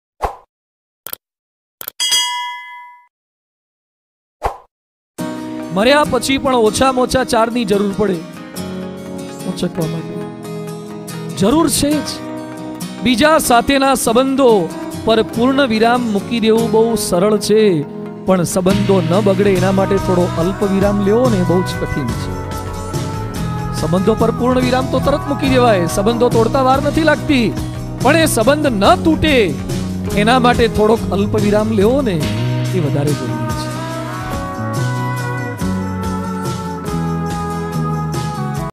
સંબંઘોમાં અલ્પવિરામ કે પૂર્ણવિરામ? WhatsApp Status by ||Apurvamuni Swami-BAPS
this video is based on how to maintain relation.
many people are says that they don't required anyone for helping hi.is this true?. DESCRIBED BY APURVAMUNI SWAMI
Apurva muni swami is a great indian motivational speaker.he lives at Rajkot BAPS swaminarayan temple.

Keep Sharing. Enjoy Watching The videos.